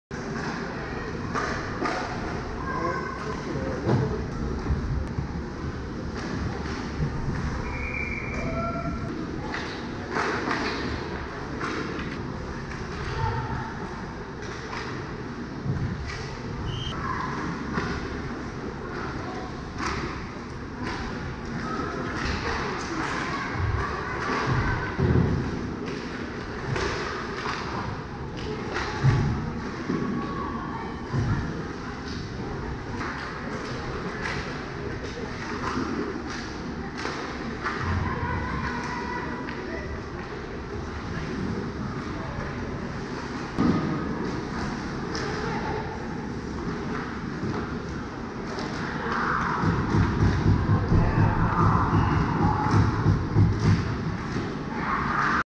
Vaughn | 12-Year-Old Hockey Goalie | GoPro
Highlight film at Pineville Ice House in Pineville NC.  half ice